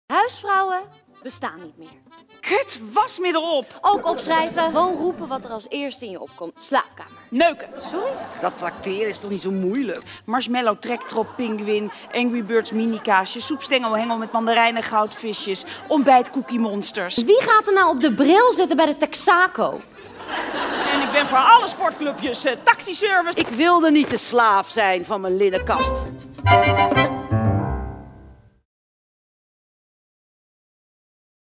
De Huisvrouwmonologen - Henriëtte Tol e.a., dinsdag 21 mei in het Isala
Er komt steeds iets bij, maar er gaat nooit iets af. Een humoristische en herkenbare voorstelling over 'alles goed willen doen'; gezin, carrière, opvoeden, netwerken en 10-minutengesprekken. Geschreven door Sylvia Witteman, gebaseerd op 150 interviews met vrouwen van toen, nu en morgen. In deze humoristische voorstelling houden we elkaar een spiegel voor, delen we onze ervaringen; frustraties; overwinningen en zetten we elkaar op een voetstuk (uiteindelijk moet iemand dat doen!)